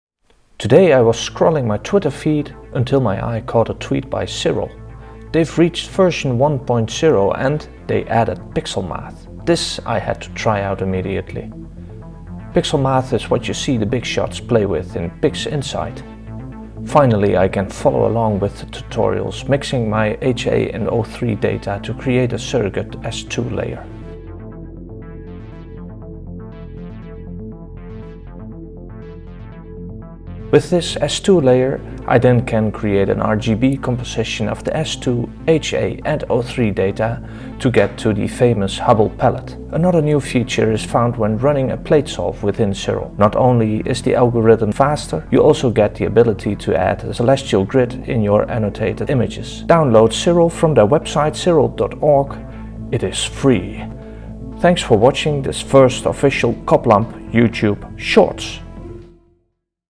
Free astrophotography processing software Siril reached version 1.0 and now has Pixel Math!
Siril reached version 1.0.

I was playing around with it and decided to quickly make a YouTube video highlighting two new features

1. Pixel Math.
Now we can play along with the big shots using PixInsight. A first version, but this is very cool (at least I think so)

2. Better Astrometry
This version has an improved Astrometry engine. Among many other things, it can now add a celestial grid in your annotated images.